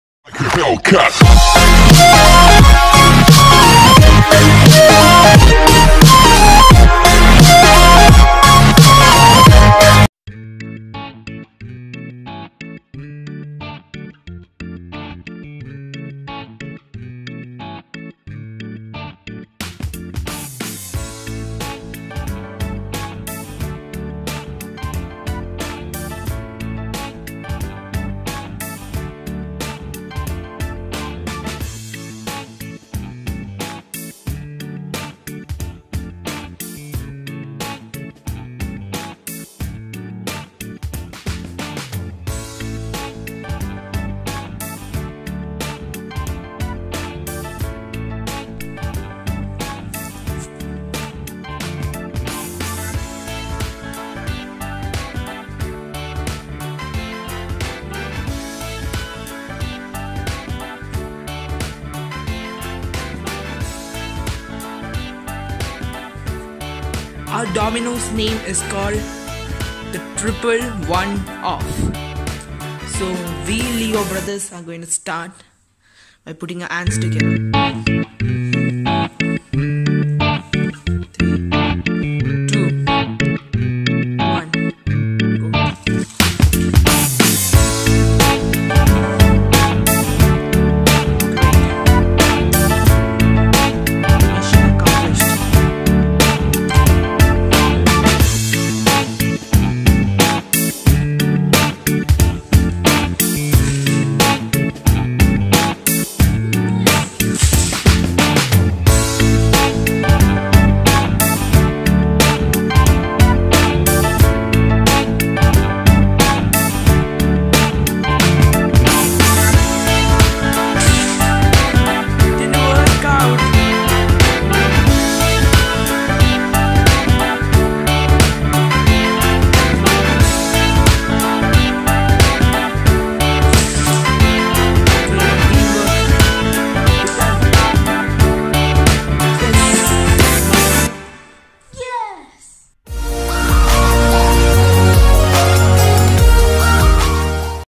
Domino stack (THE BROS)
This video is made by us to inspire you guys to not waste time during free time you can do such activites like domino stack to increase your creativity. Enjoy!!!